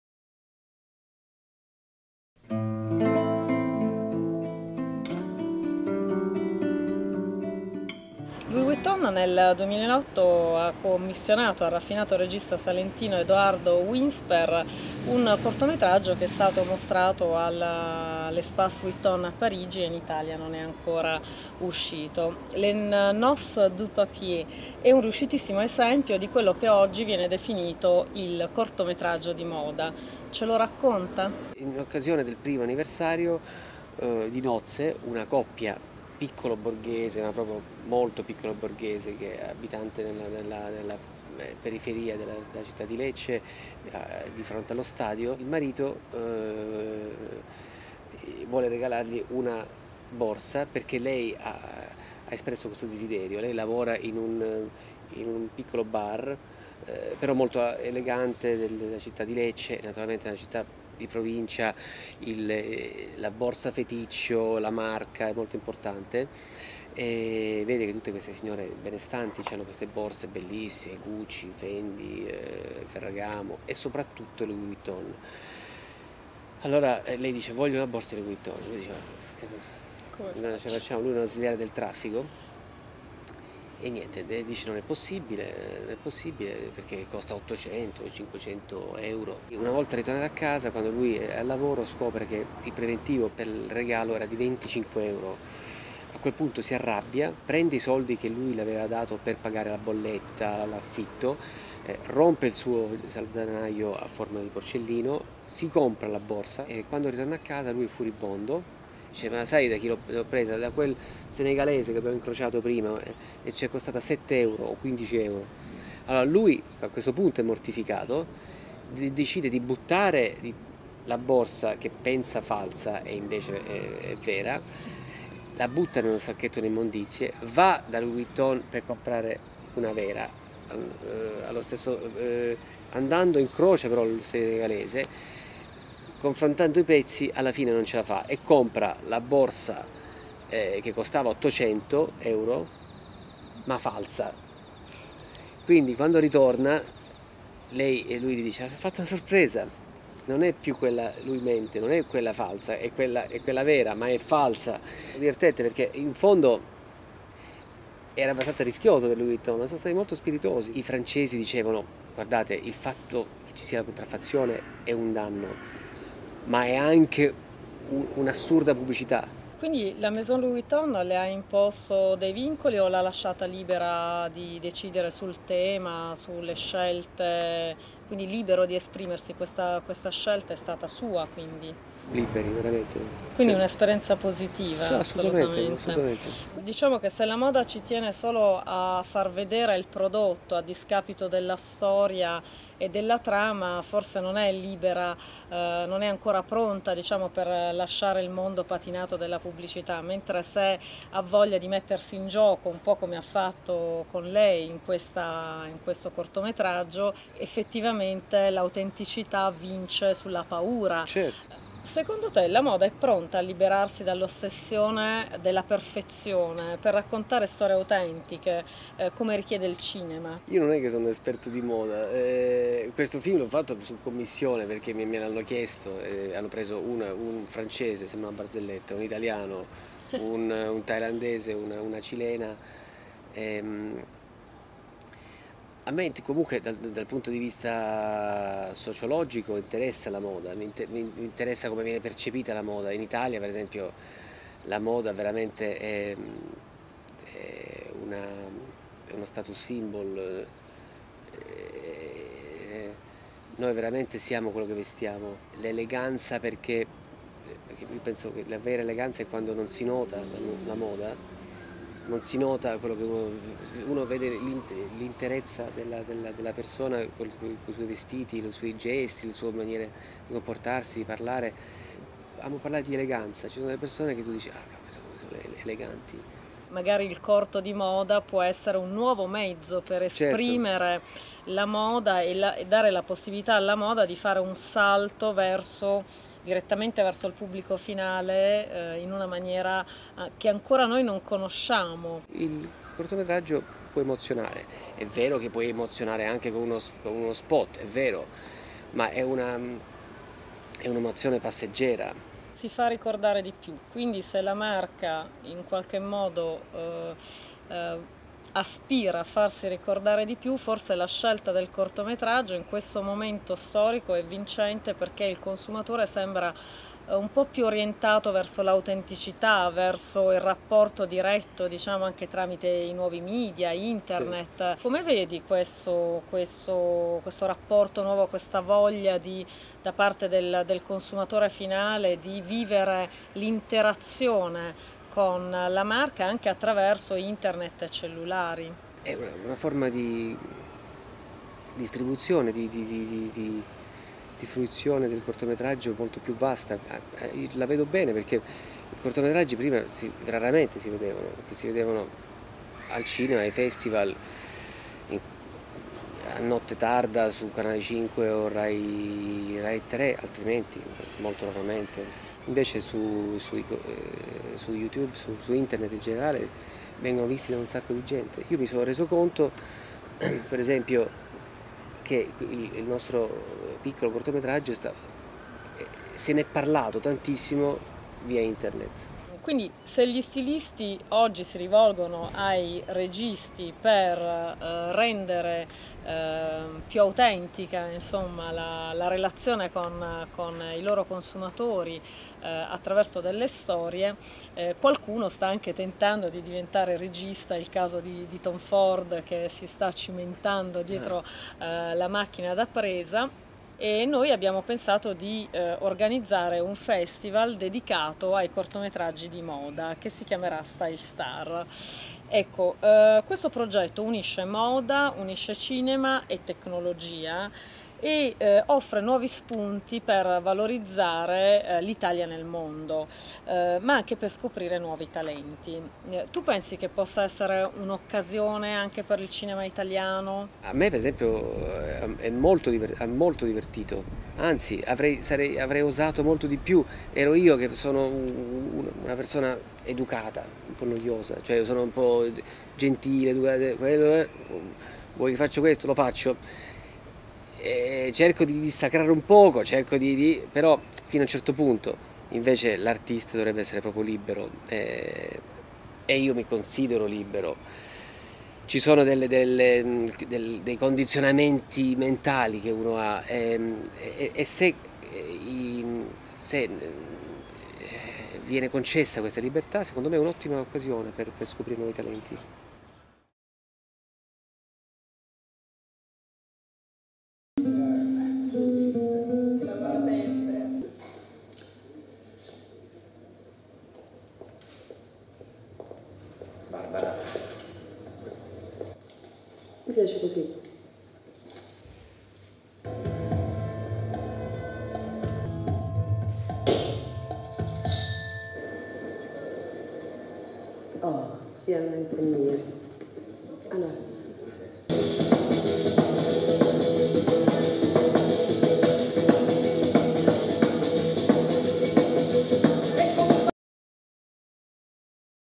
Intervista a Edoardo Winspeare per il nuovo corto di LOUIS VUITTON "Il primo anniversario"
da Marina Garzoni/ Style Star.

Edoardo Winspeare ci racconta il nuovo corto di moda da LOUIS VUITTON.

Regia: Edoardo Winspeare.
con: Barbara De Matteis.